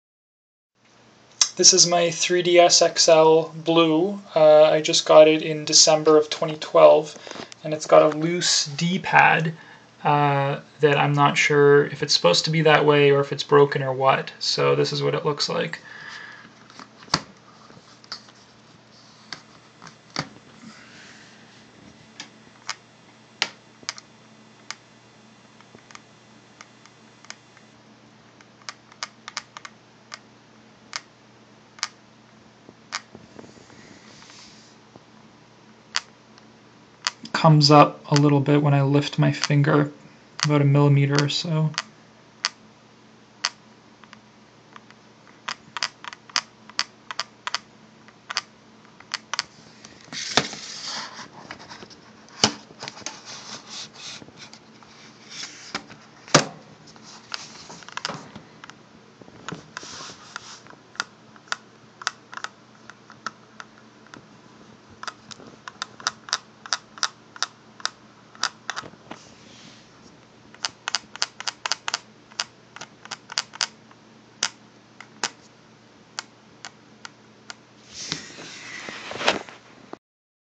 Nintendo 3DS XL blue - loose D-Pad
Got a new Nintendo 3DS XL (blue) for December 2012 and noticed that the D-pad seems loose. It jiggles about a bit in its slot and when I rest my finger on it then lift it, the d-pad also lifts by about a milimeter or so. Not sure if this is normal or if it's been assembled improperly so I thought I'd post this video to get others' opinions. What's your D-pad like - better? Worse? Thanks!

NOTE: Video loses sync 1/3 of the way through so the clicking you hear is delayed by a second.

DEC 29 '12 UPDATE
Just returned my 3DS XL to the Best Buy that I bought it at (Yonge/Dundas in Toronto)... and it's got the exact same problem! Which, it seems, is not a problem at all but how it should be. I guess that settles it. Now I just feel like a jerk for having panicked and "wasted" the return over nothing.

FEB 04 '13 UPDATE
Have heard from several people and it seems this is a widespread thing for any XLs purchased in the last year. It's not defective - it's just how it is. Enjoy your 3DS!